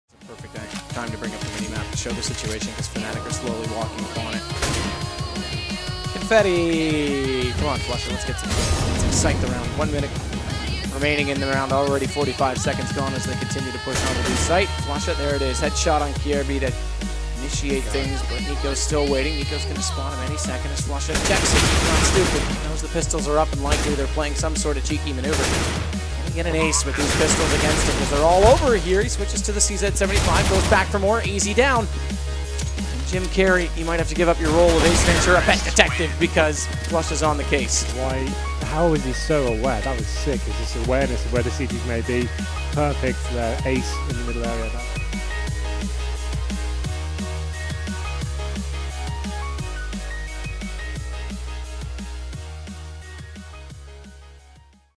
[Frag Clip] Ace "Flusha" Ventura
This round show just how insane Flusha's gamesense is!

Editing Gear:
Video Card: R9 295X2
SSD: Crucial 500GB
CPU Cooler: Be Quite Dark
CPU: Intel i5 4690K
Motherboard: GA-Z97X-SLI

Casting //
Thanks to PGL